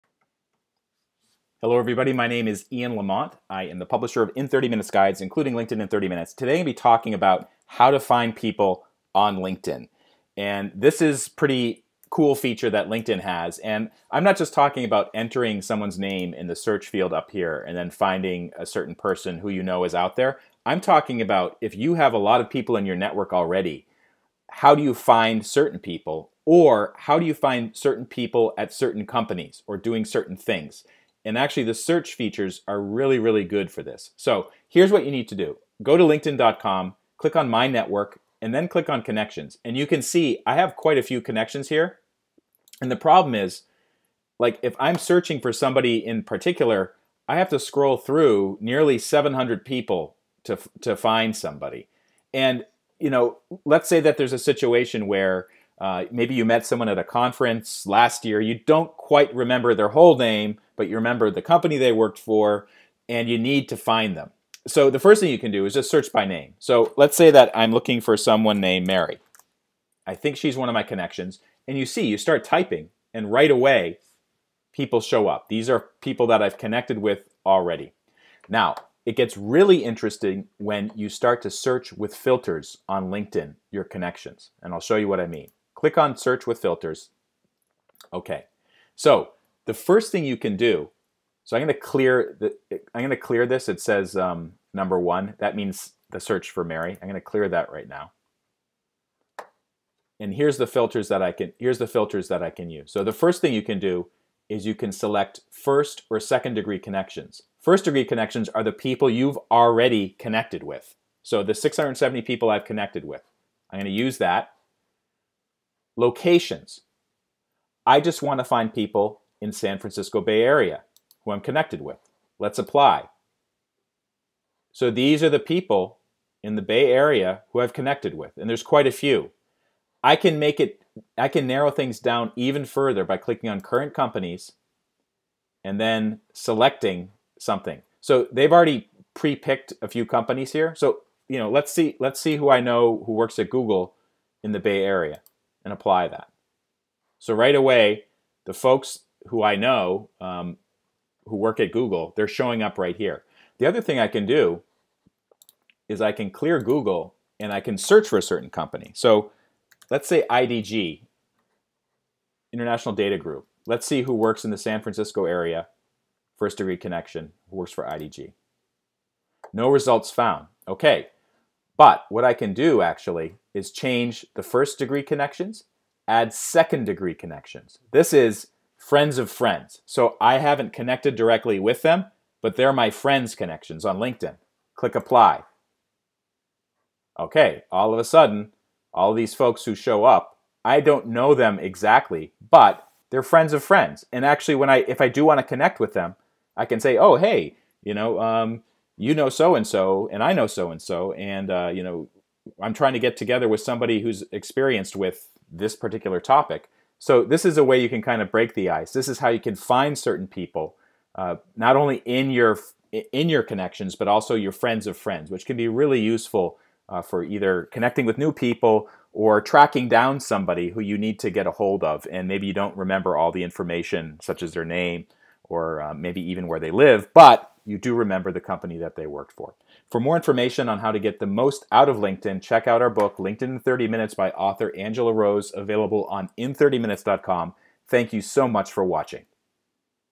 Quickly find & filter LinkedIn connections
If you have a lot of people in your LinkedIn network, you may want to find someone quickly but don't remember their name, only their company or location. In 4 minutes, learn how to use filters to find people in your LinkedIn network, as well as 2nd degree LinkedIn connections, using partial names, companies, and locations. Narrated by the publisher of IN 30 MINUTES guides, including LINKEDIN IN 30 MINUTES.